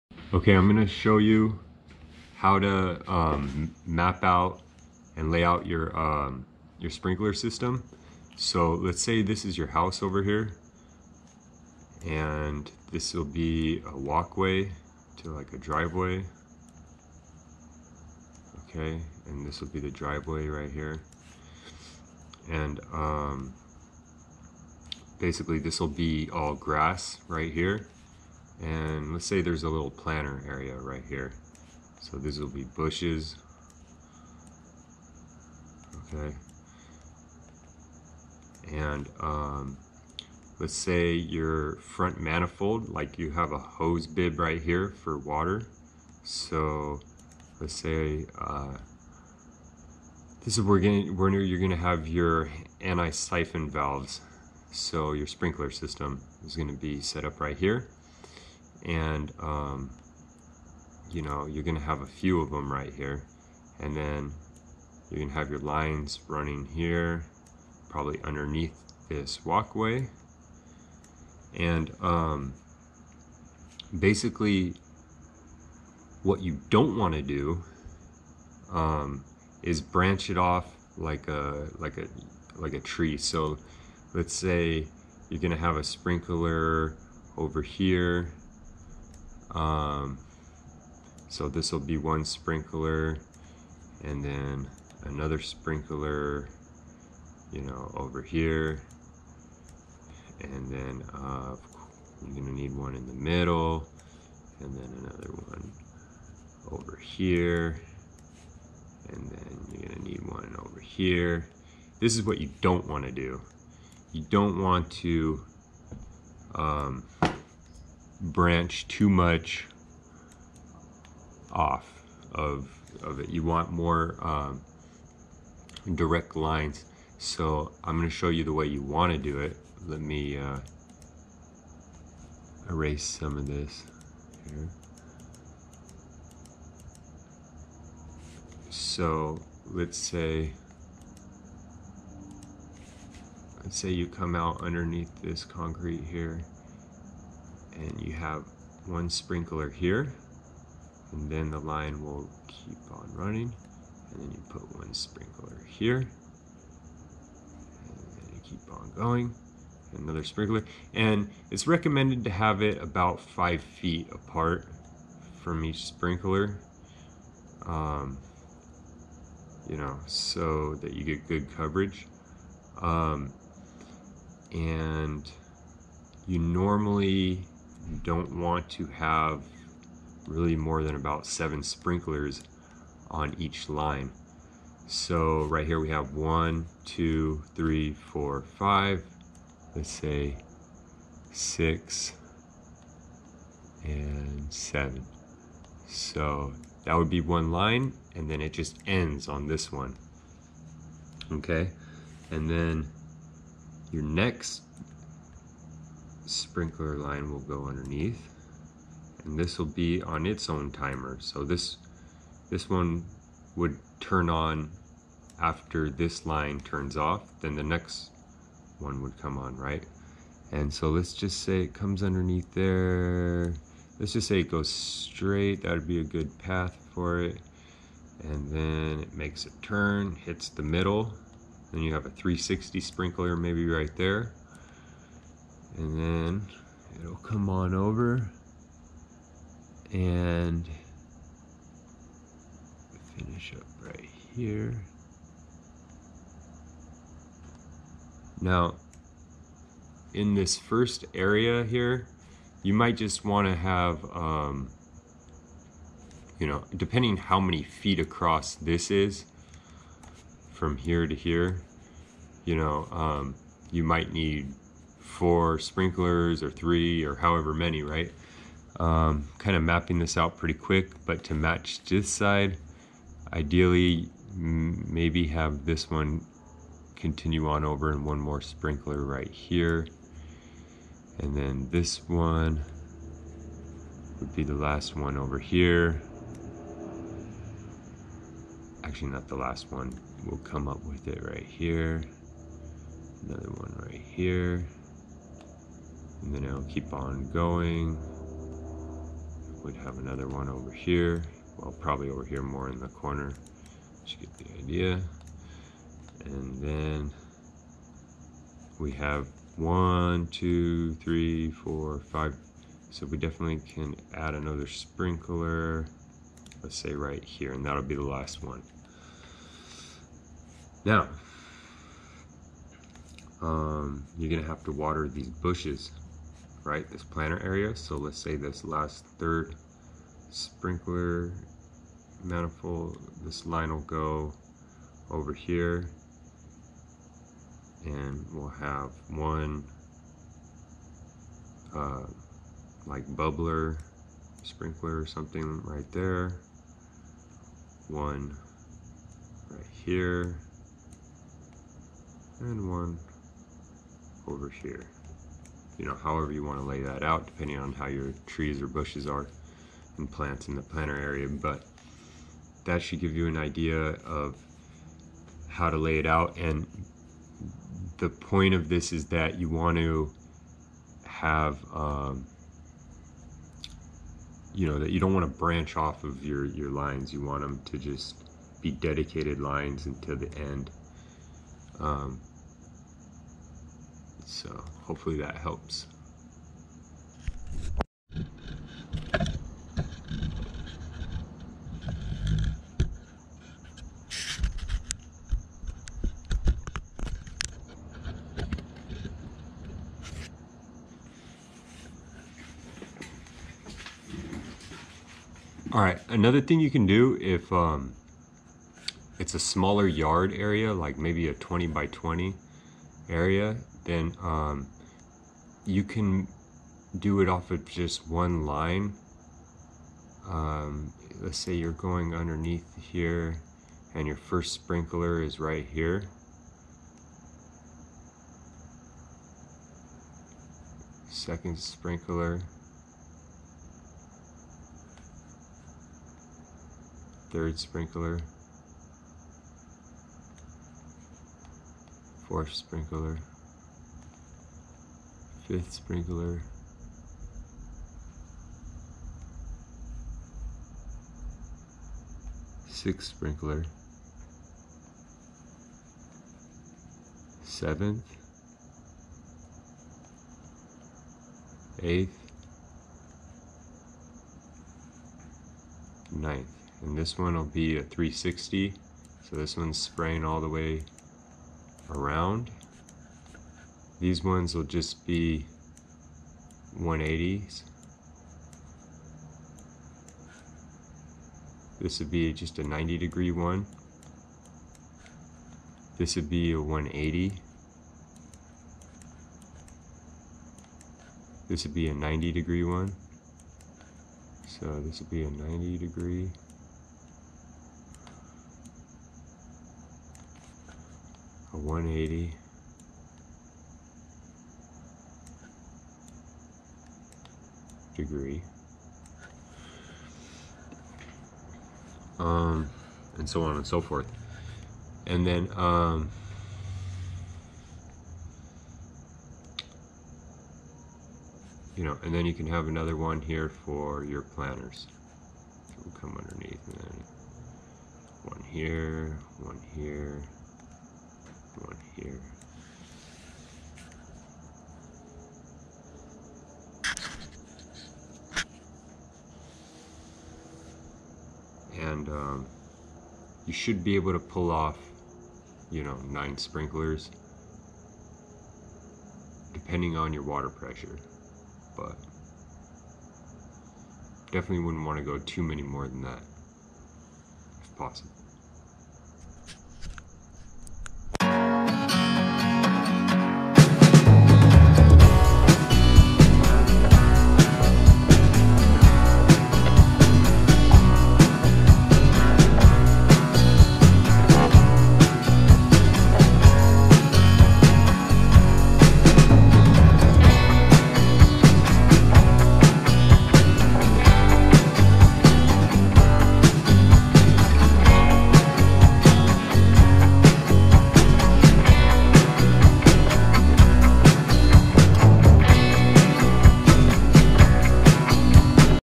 How to map out a sprinkler system
this video shows how i map out a sprinkler system